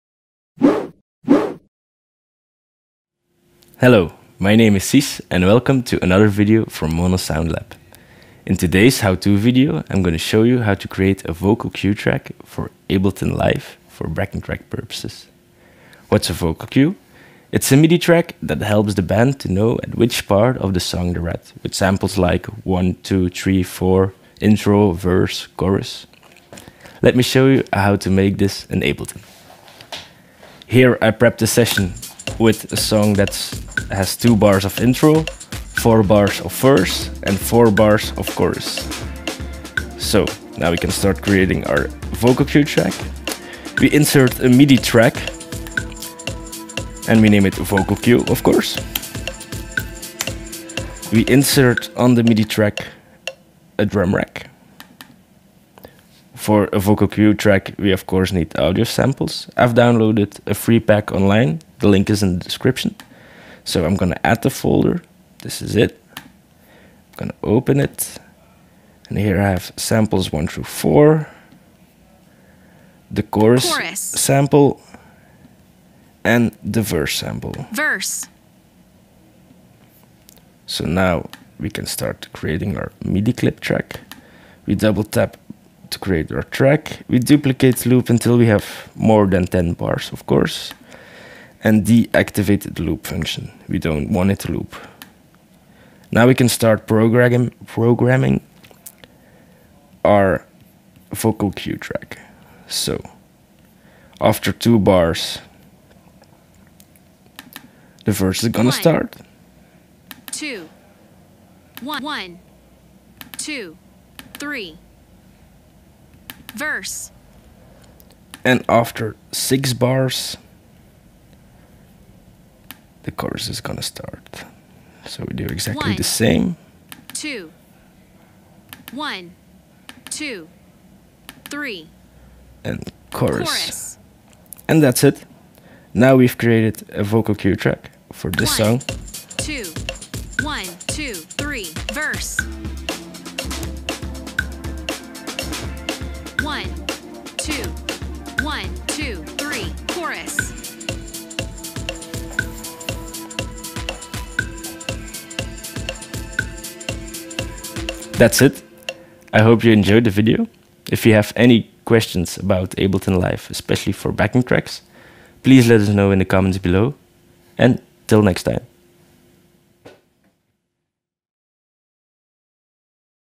How to create a vocal cue track in Ableton Live
Cis from MONO SoundLab explains how to create a vocal cue track in Ableton Live.

If you want us to cover any specific topic regarding pro audio, leave a message in the comments!